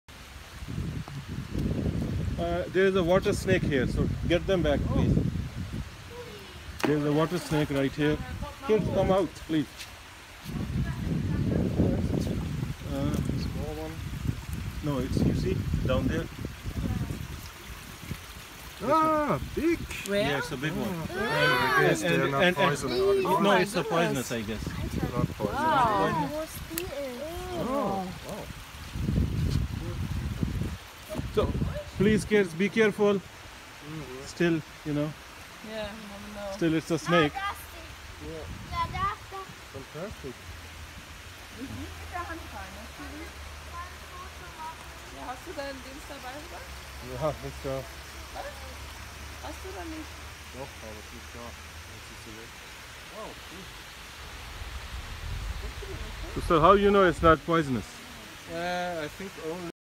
Spotted Water Snake in Ithaca, NY
Imam Sahib Spotted water snake in this beautiful natural fall area in Ithaca, NY, USA